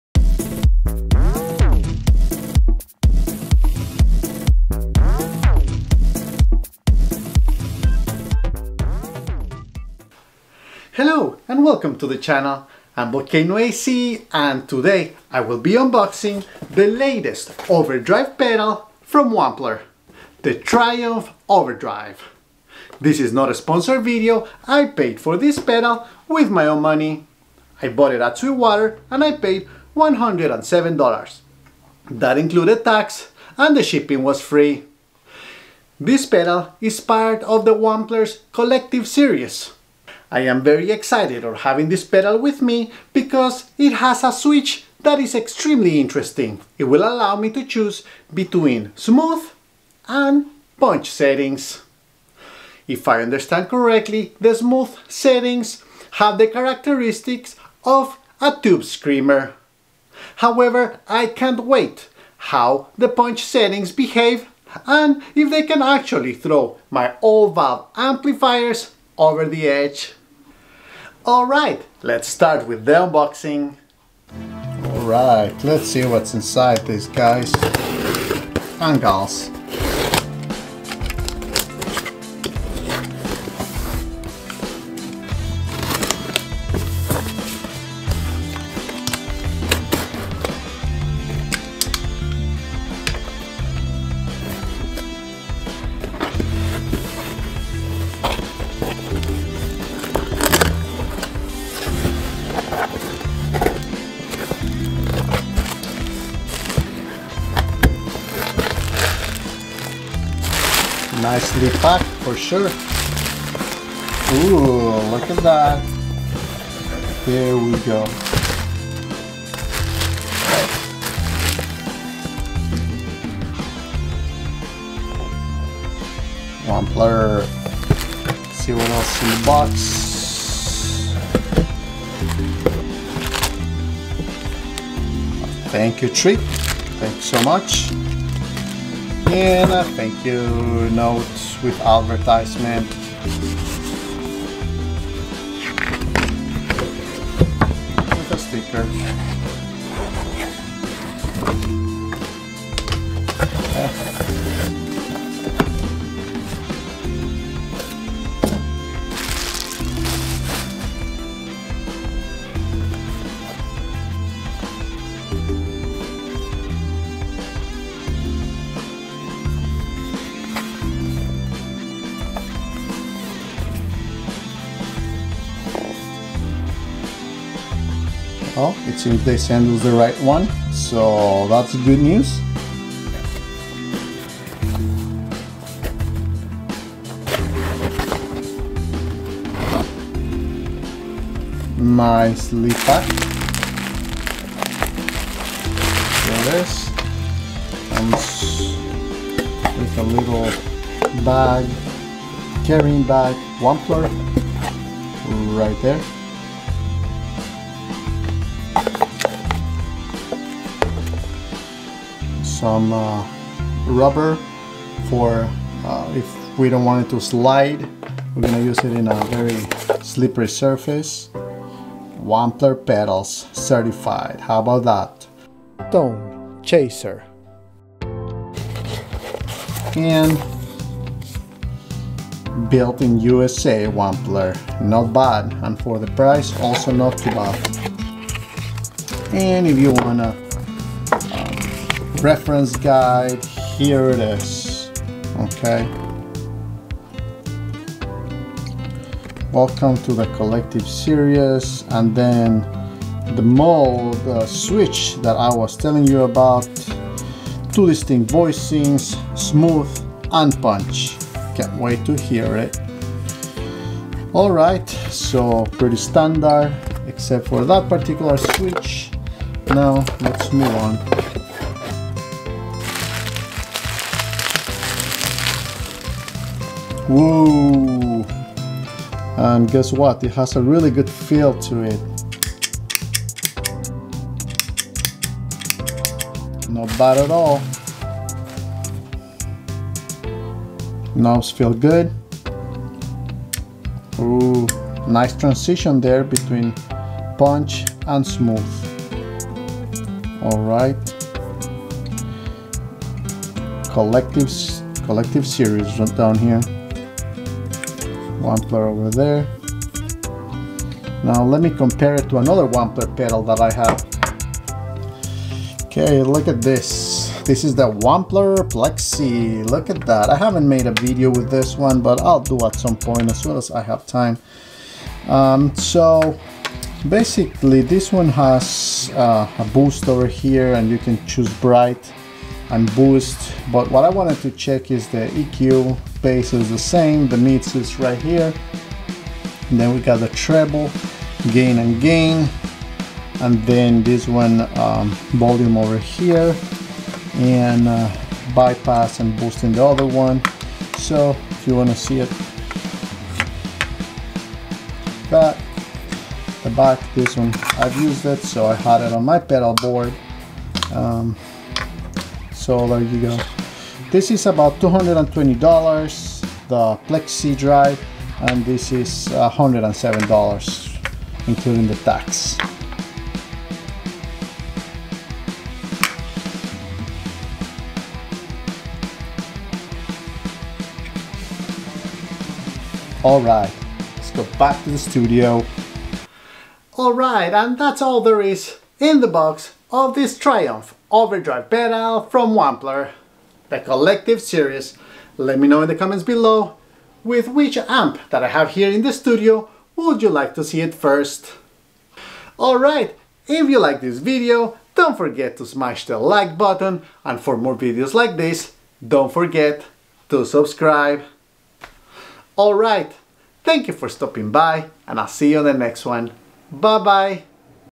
Unboxing the Triumph Overdrive pedal, the newest overdrive pedal from Wampler. Collective series.
In this video I unboxed the new  the Triumph Overdrive pedal, the newest overdrive pedal from Wampler. It is part of Wampler's Collective series.
I purchased this pedal with my own money, this is not a sponsored video.